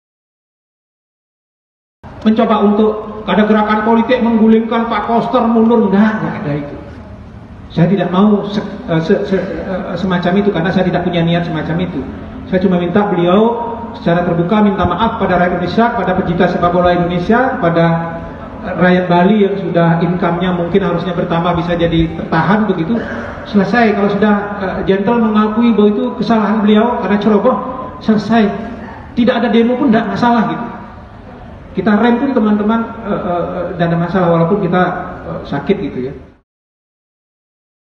Gubernur Bali Dituntut Minta Maaf
Liputan6 - Gubernur Bali Dituntut Minta Maaf

Sejumlah kelompok pecinta sepak bola di Bali menuntut Gubernur Bali I Wayan Koster untuk meminta maaf usai Indonesia dipastikan batal menggelar Piala Dunia U-20 2023.